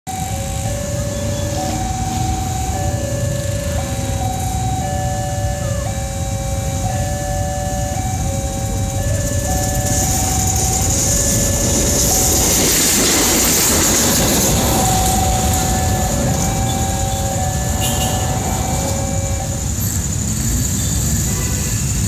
Seruntulan Kereta Api Ukur Jalan Rel
Kereta Api Ukur Jalan Rel berjalan seruntulan  menuju stasiun Bandung, melintasi palang pintu Cimindi 11/03/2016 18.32